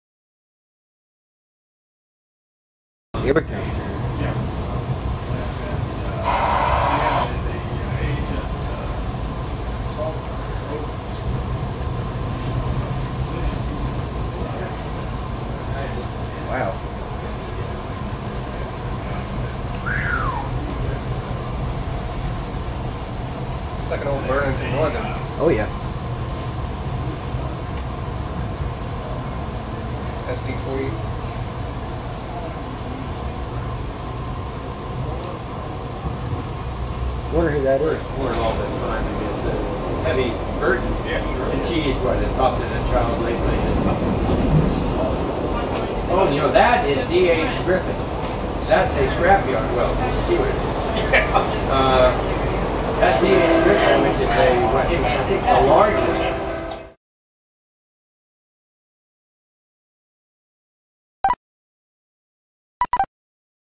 Amtrak 73 Meeting NS 159 with a CEFX SD40-2 & Amtrak 80 in Greensboro NC 3/17/2012
Running a tad behind Amtrak 73 meets NS 159 with an interesting set of power an SD70M-2 leader, the ubiquitous GE D9-40CW, and a rare CEFX SD40-2. Latter on we see Amtrak 80 in Hilltop waiting for 73 clear on by.
Amtrak 73
NCDOT EMD F59PH #1869
NS 159 Greensboro/Elm
NS EMD SD70M-2 #2755
NS GE D9-40CW #9555
CEFX EMD SD40-2 #7120 Ex BN
Amtrak 80 Hilltop
Amtrak GE P42DC #24